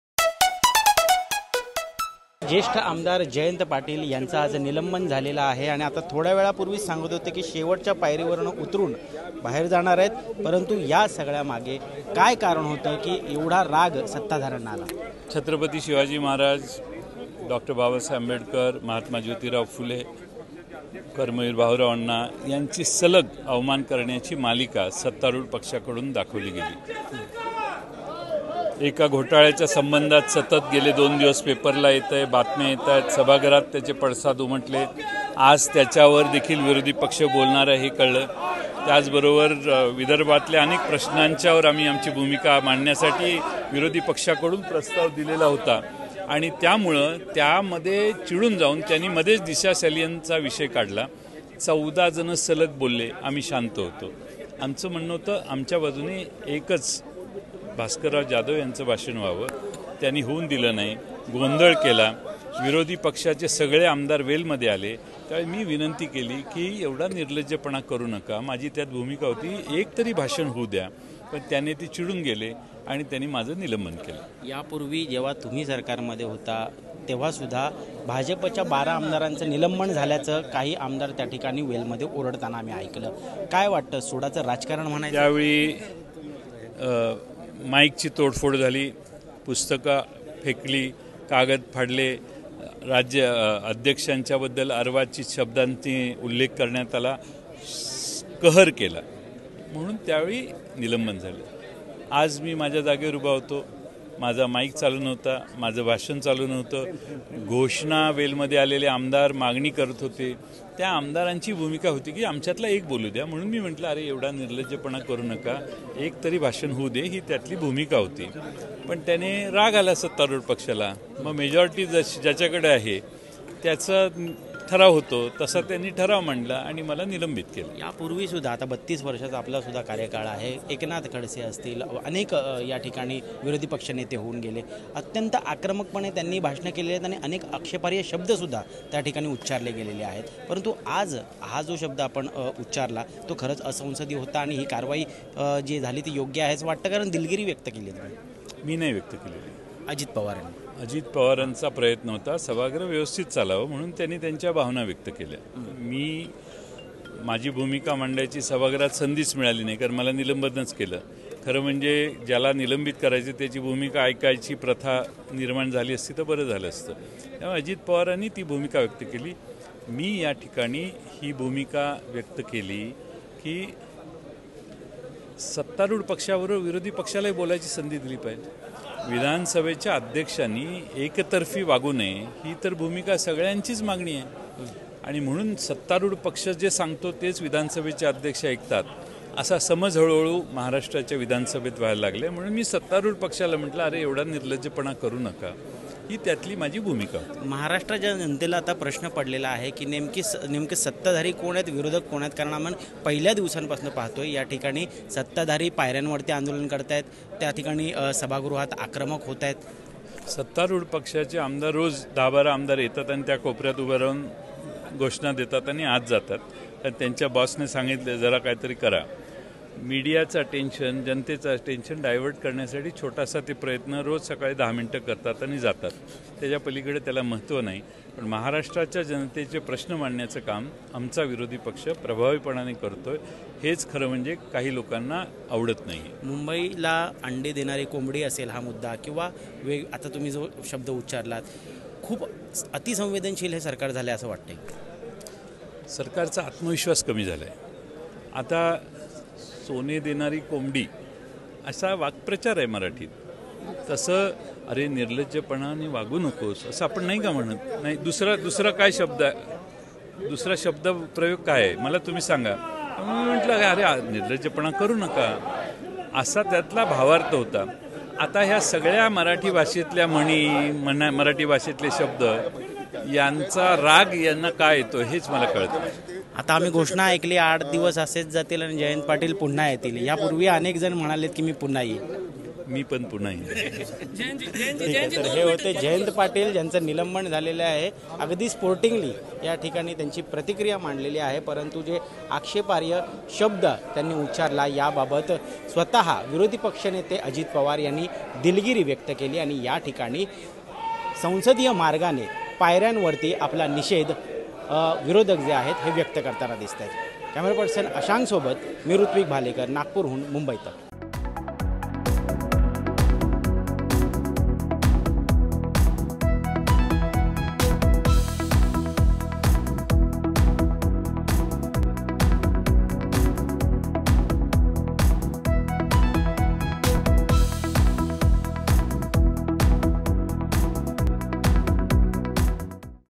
Vidhan Sabha : Jayant Patil यांनी विधानसभा अध्यक्ष राहुल नार्वेकर यांना निर्लज्जपणा शब्द का वापरला?
जयंत पाटलांनी विधानसभा अध्यक्ष राहुल नार्वेकर यांना ‘निर्लज्जपणा’ शब्द का वापरला?